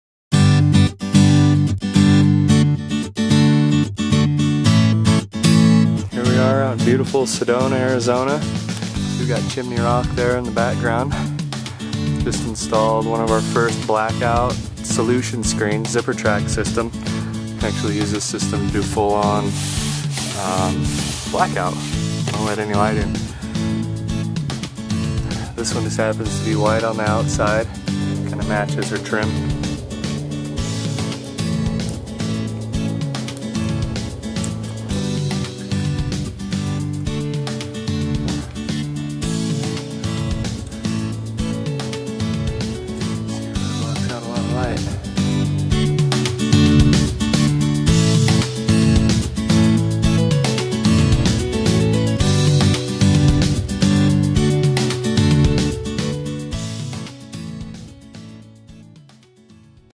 Exterior Window Shade: Complete Blackout Installation
Full blackout application for this client’s home in Sedona, Arizona utilizing a Motorized Zipper Track High Tension Screen with Blackout Material option.  Provides complete sun screen protection and privacy.